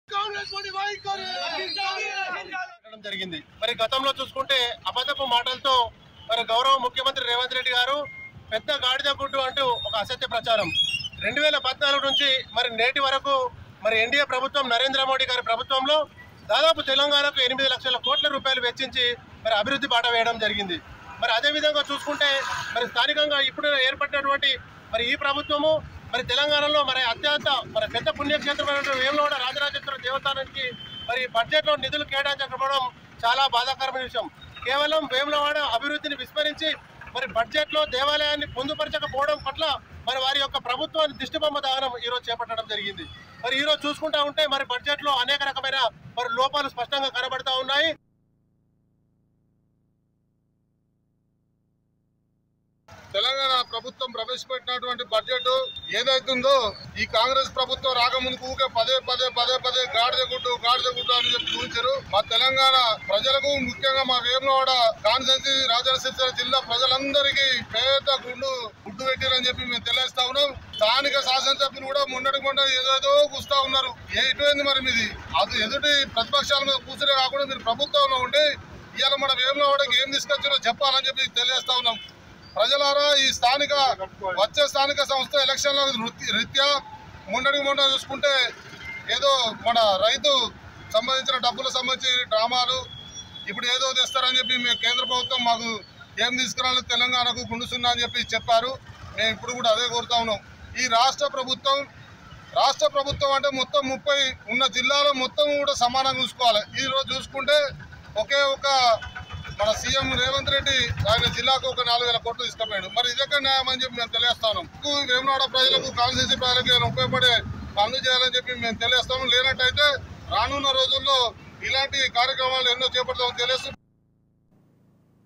వేములవాడ ఆలయానికి నిధులు నిల్ | Vemulawada Bjp Activists Protest over On Cm Reventhreddy | Raj News
వేములవాడ ఆలయానికి నిధులు నిల్ | Vemulawada Bjp Activists Protest over On Cm Reventhreddy | Raj News Telugu